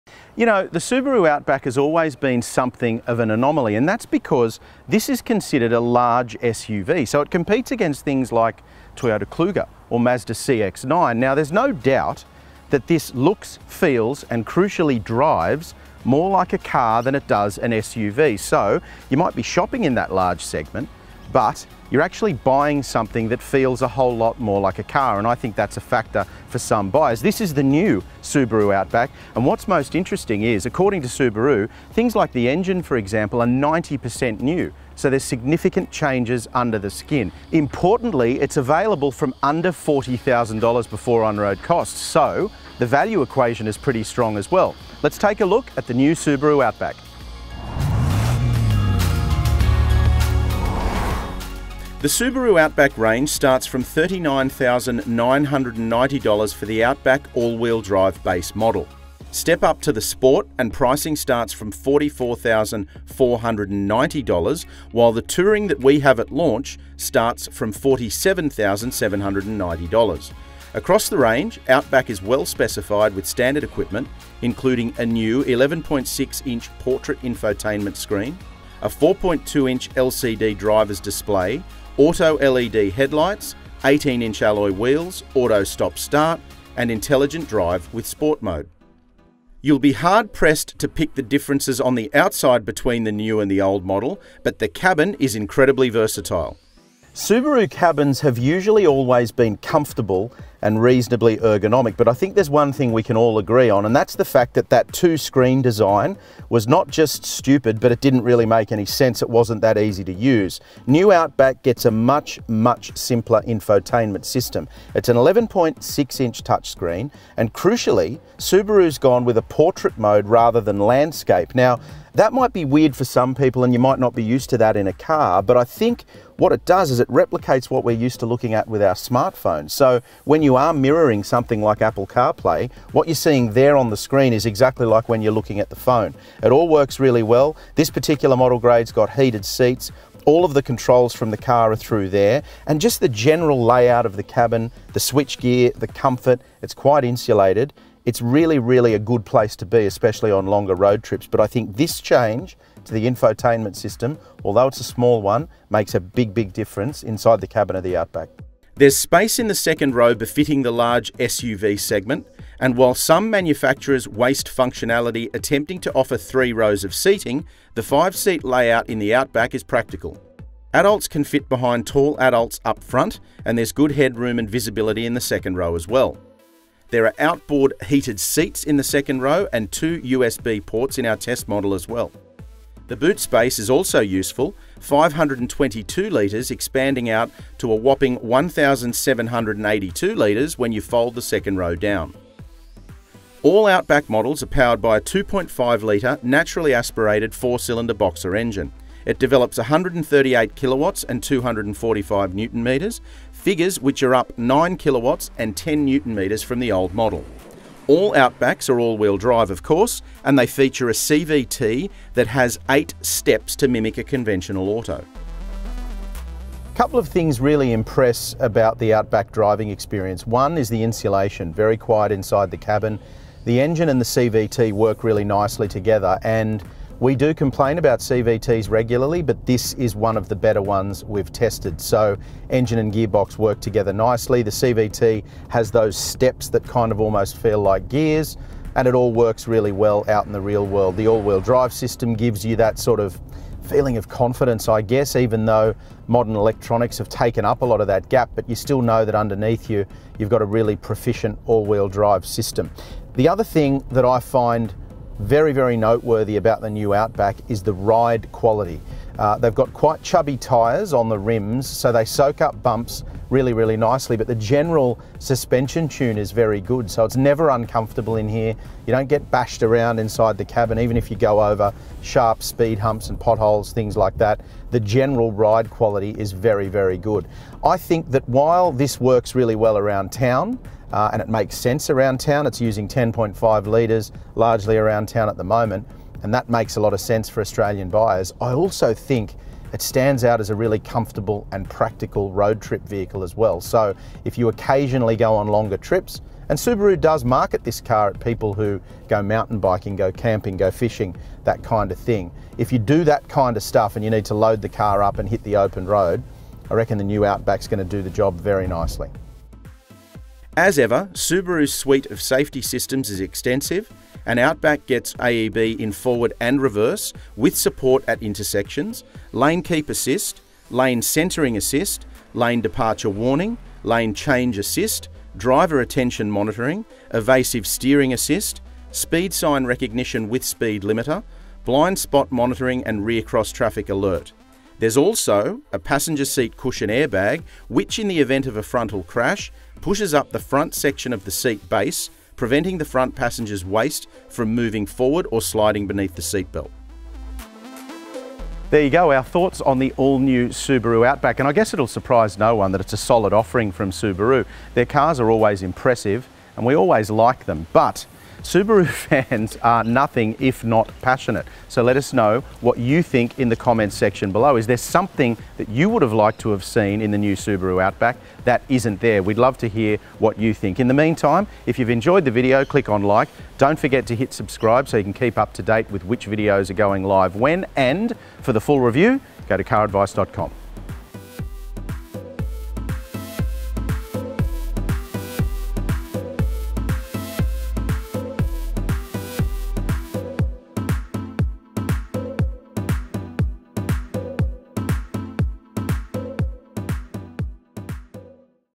2021 Subaru Outback First Drive Review | CarAdvice
The large-SUV segment is an interesting one, with everything from FWD wagons and rugged 4WDs to the much more car-like 2021 Subaru Outback vying for buyers’ attention. While vehicles in this segment ideally need to be a jack-of-all-trades as part of the brief, few suit the urban dweller as comprehensively as the Subaru Outback.